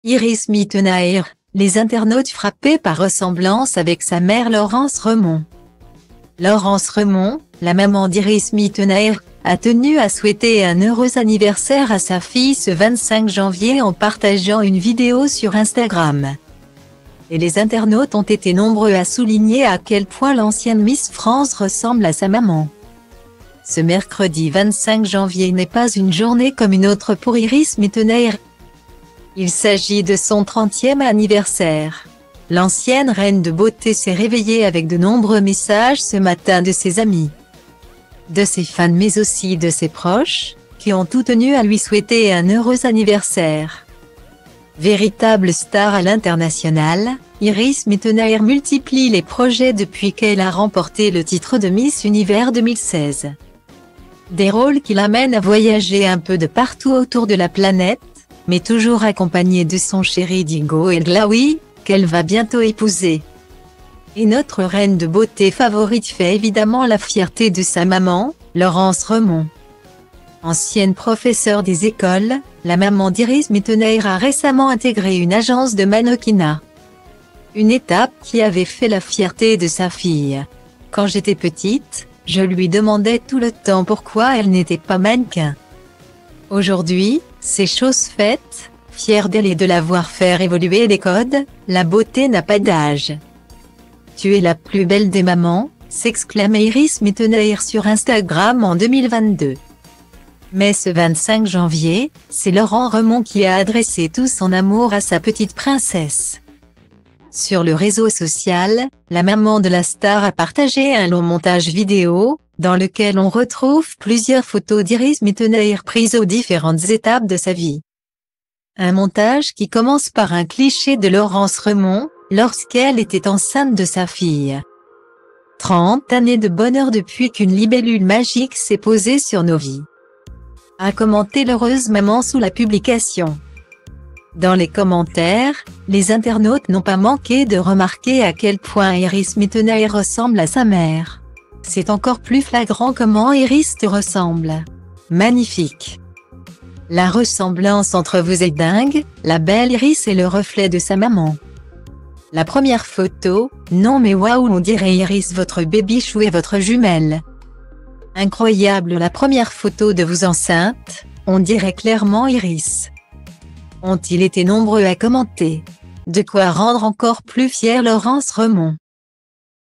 Iris Mittenaere : les internautes frappés par ressemblance avec sa mère Laurence Remond
Laurence Remond, la maman d’Iris Mittenaere, a tenu à souhaiter un heureux anniversaire à sa fille ce 25 janvier en partageant une vidéo sur Instagram.Et les internautes ont été nombreux à souligner à quel point l’ancienne Miss France ressemble à sa maman. Ce mercredi 25 janvier n’est pas une journée comme une autre pour Iris Mittenaere.